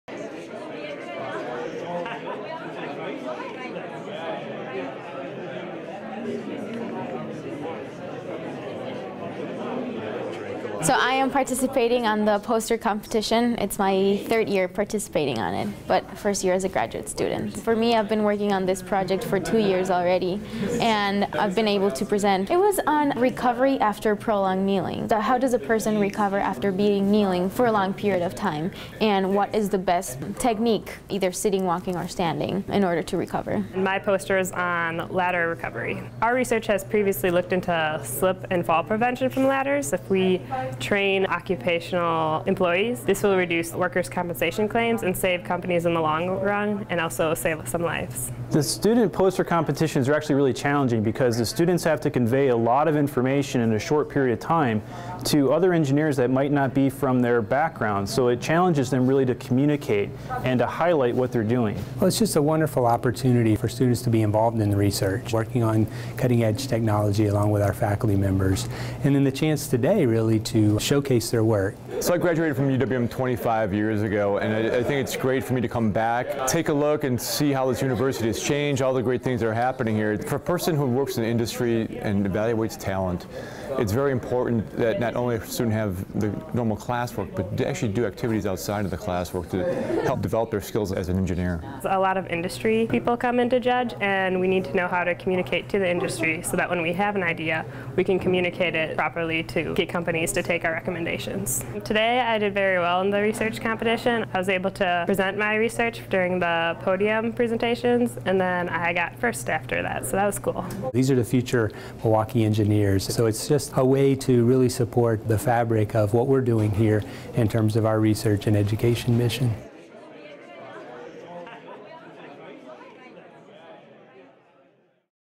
2015 Student Research Poster Competition
Watch as more than 50 UWM College of Engineering & Applied Science undergrad and graduate students present their research with thousands of scholarship dollars on the line. Hear from students, alumni, faculty, industry partners and College of Engineering Dean Brett Peters about this phenomenal annual research and networking opportunity at UWM. Learn more about this year's research, winners and judges here: